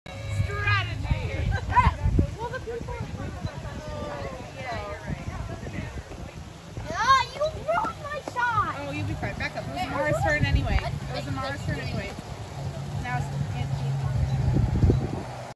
Glow Golf
We had an incredible night for our first ever Glow Golf event! On May 26, 2021, over 60 people came to putt in the dark with LED balls, glow sticks, a glow-in-the-dark course. . . all under the stars. We even got to see the coolest full moon!